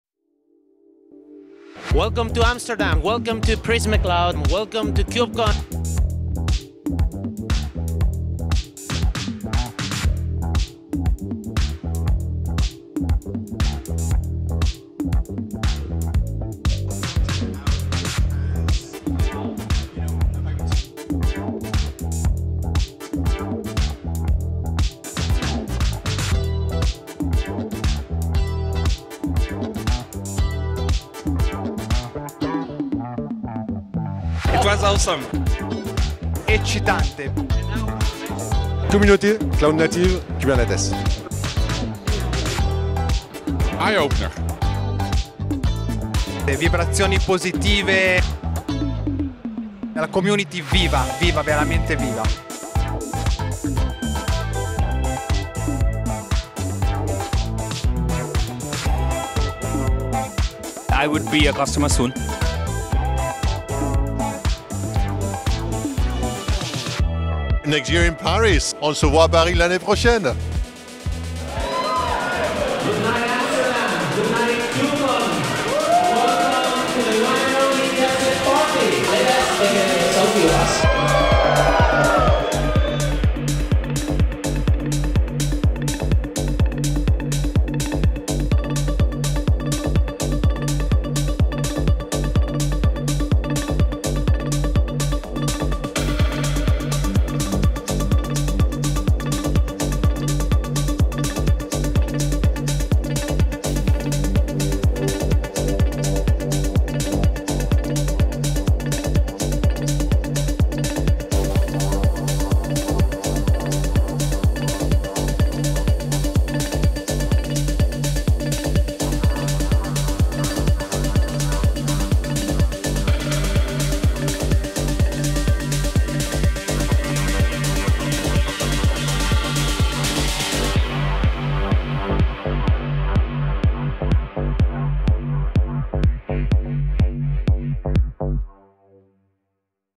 Prisma Cloud at KubeCon + CloudNativeCon Europe 2023
KubeCon + CloudNativeCon Europe 2023 ended and the Prisma Cloud team enjoyed great moments with all attendees! We had many interesting conversations at our booth about the cloud-native security topics and a wonderful night at the DevSec party in Amsterdam. We hope to see you soon in the next conference!